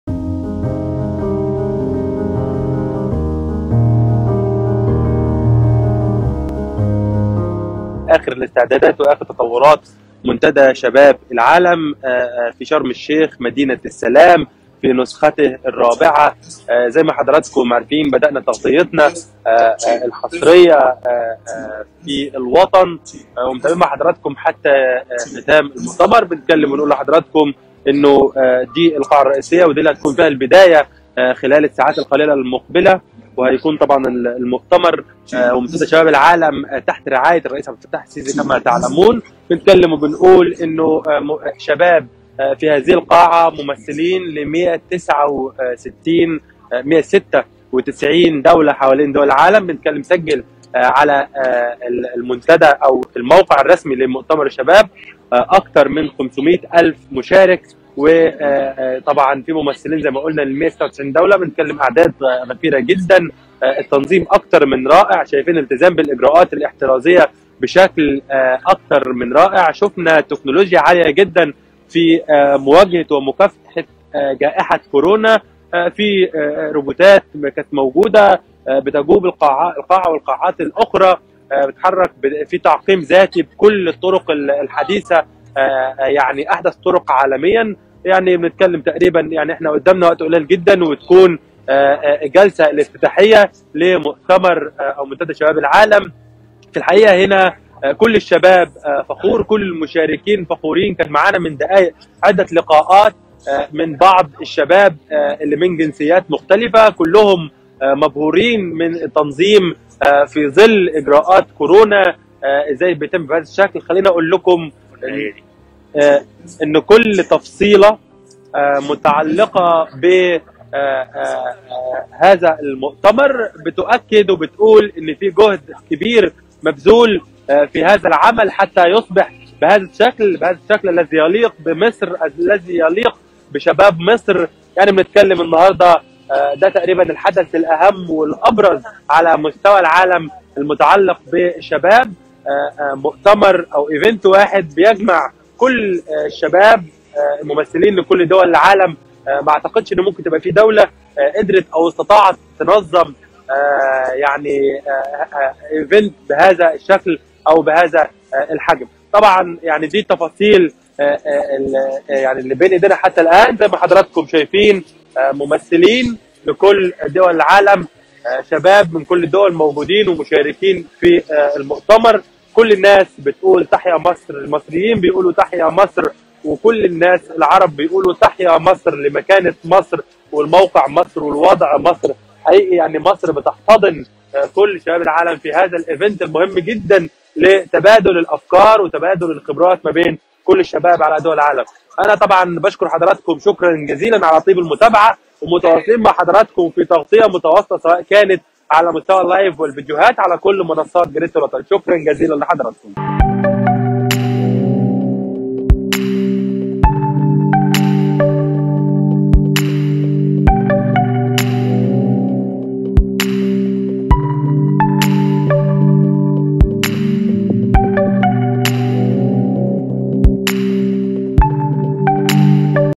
استعدادات مكثفة لافتتاح منتدى شباب العالم بمدينة السلام
استعدادات مكثفة لافتتاح منتدى شباب العالم بمدينة السلام.. تكنولوجيا حديثة لتأمين المشاركين